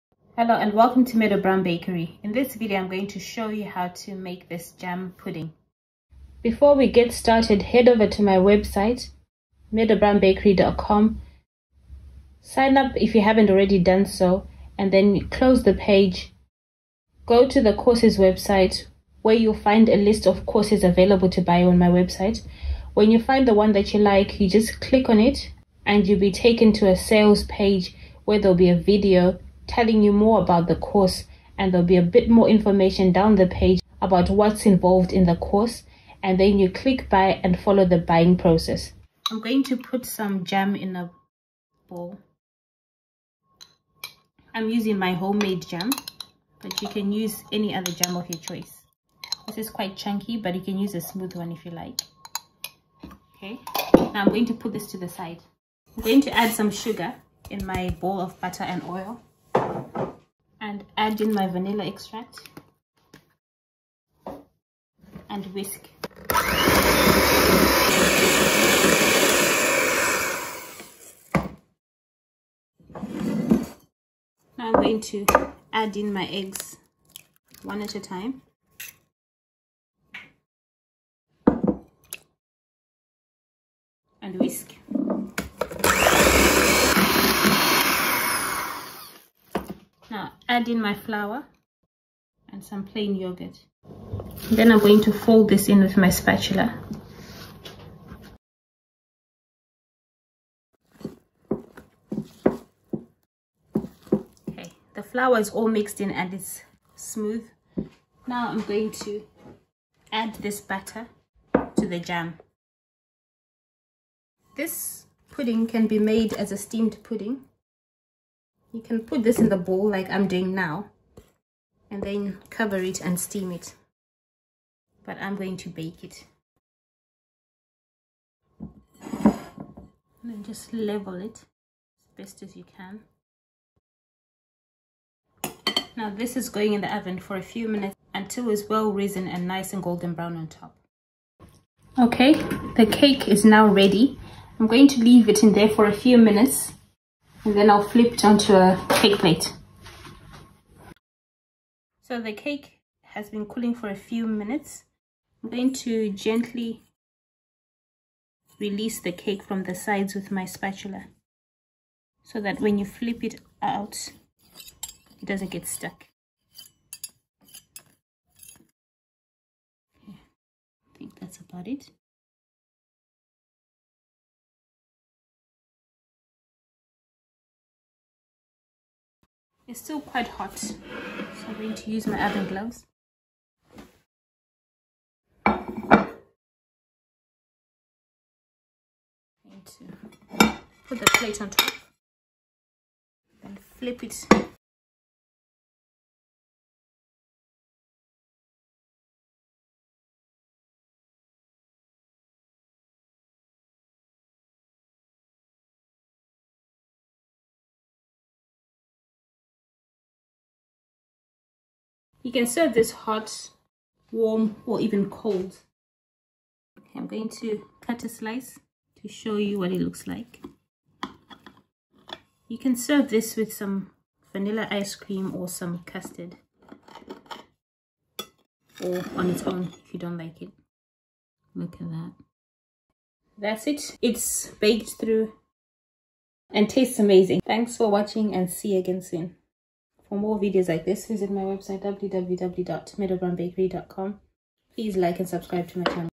How to make a steamed pudding in the oven : Baked jam sponge pudding UK : Steamed sponge cake recipe
Step by step video tutorial showing how to make a steamed pudding in the oven. This is an easier way to make steamed pudding. It works with a steam oven or one without steam. SteamPuddingInOven 😀😍👌🏾

What is steamed pudding?
Steamed pudding is usually made on a stove top in a pot with water and brought to a simmer.  In this video I show you how to make a steamed pudding in an oven.

Unlike the traditional steamed pudding this one is made in an oven.

Can you steam a pudding in the oven?
Yes, in this video I show you how to make a steam pudding in an oven

Let’s connect:

Checkout some of the first series of paid courses for beginners on my website.  Well explained and in detail.